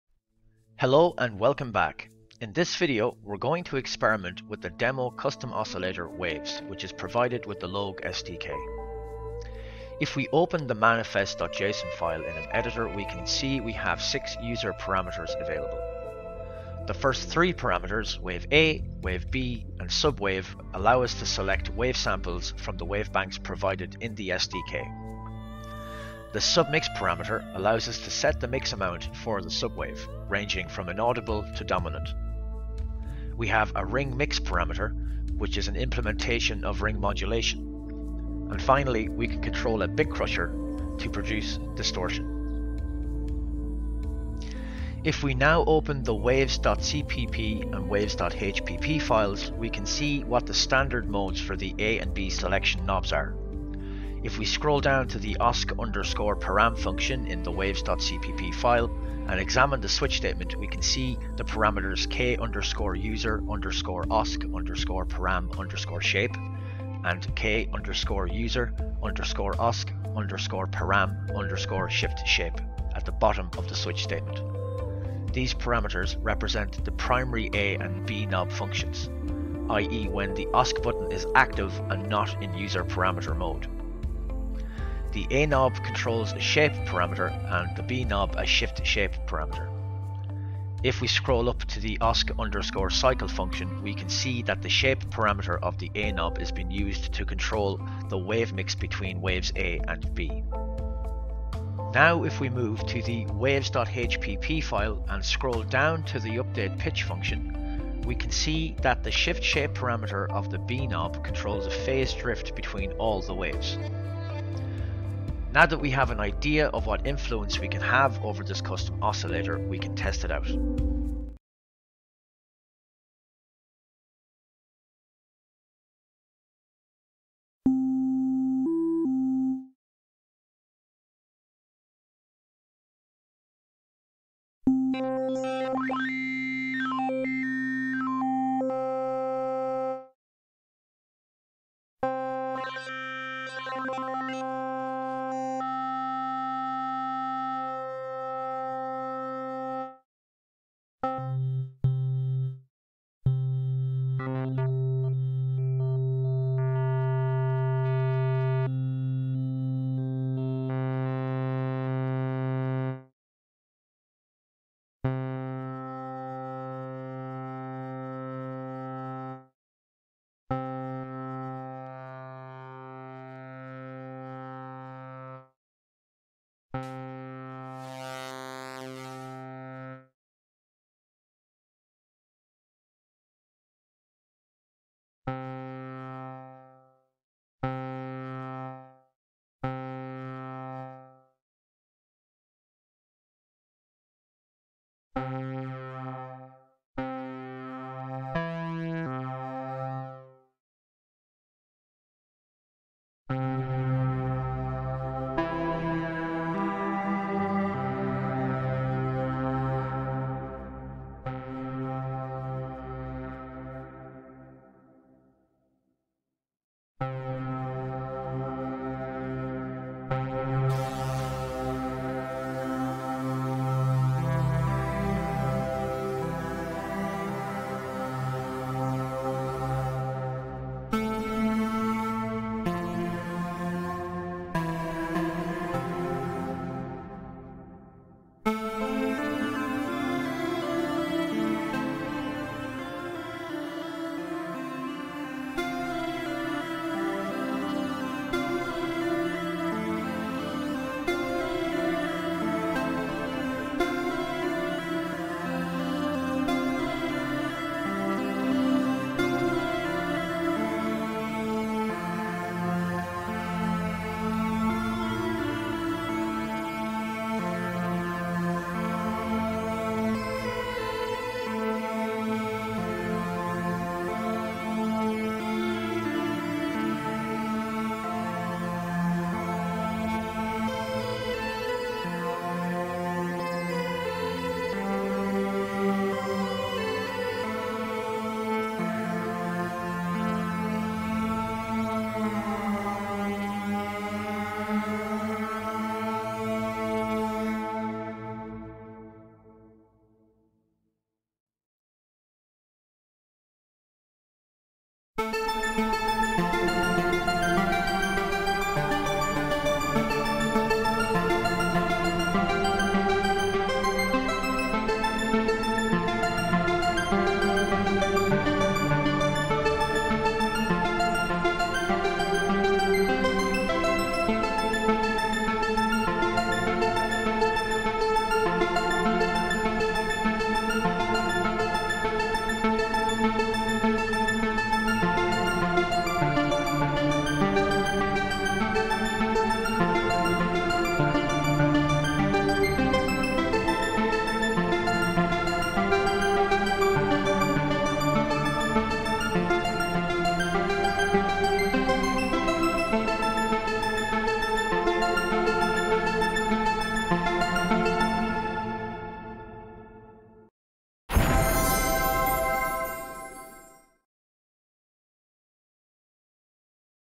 Korg NTS-1 Custom Oscillator Demo: Waves
In this video we demonstrate the custom oscillator "waves", which comes with the Logue-SDK, on the Korg NTS-1 Nu:Tekt digital synthesizer.

Thumbnail image credit: Korg